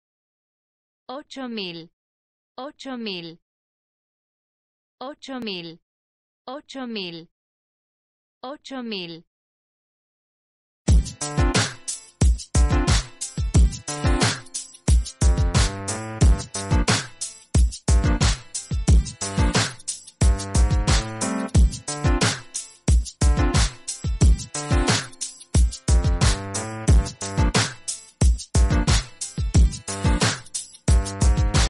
How do you say 'eight thousand' in Spanish?
How to say 'eight thousand' in Spanish? Listen to hear the pronunciation.